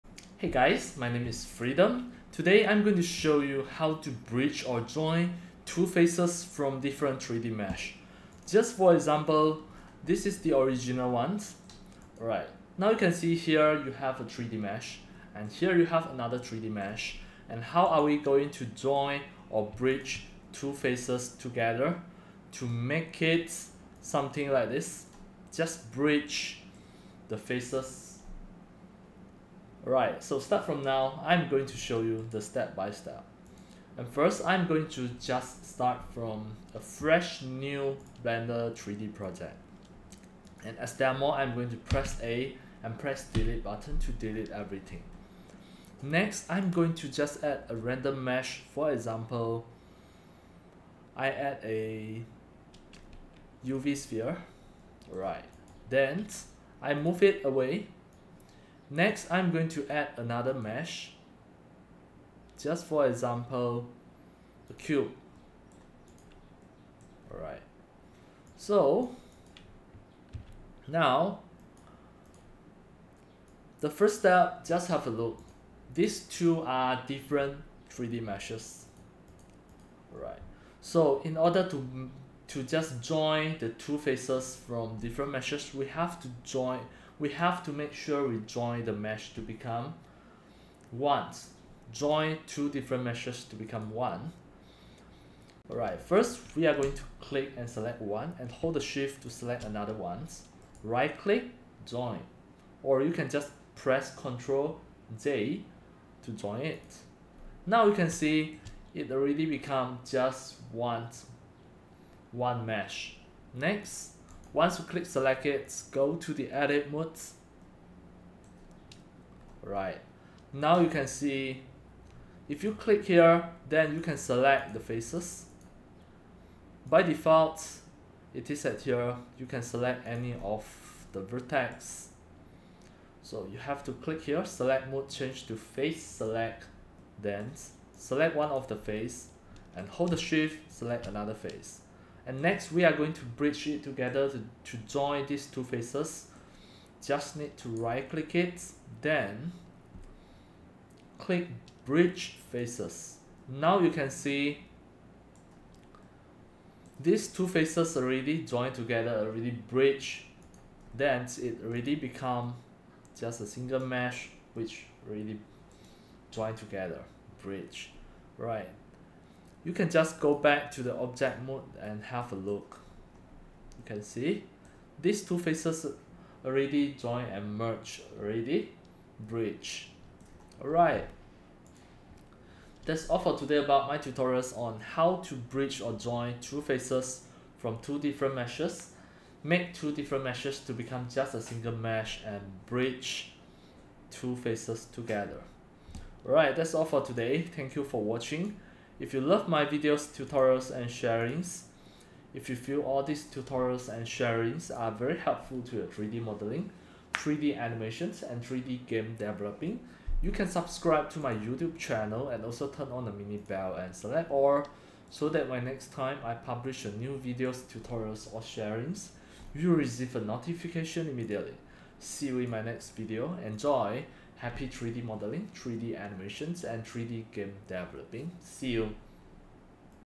Bridge & Join Faces in Blender 3D Model Meshes | Edit Mode | Tutorial
Description:
Hey guys, if you have two 3D model meshes, how to join and bridge two faces from two different 3D Meshes in Blender 4.0? This is my step-by-step tutorials on how to do this! ENJOY! and having fun! Happy 3D Modeling, 3D Animation + 3D Game Dev! ENJOY!

Enjoy! and have fun! Happy 3D Modeling, 3D Animation + 3D Game Dev! ENJOY!

Blender,
Bridge,
Face,
Join,
Edit Mode,
CGI,
3D Modeling,
3D Animation,
Game Dev,
Tutorial,
Freedom Arts,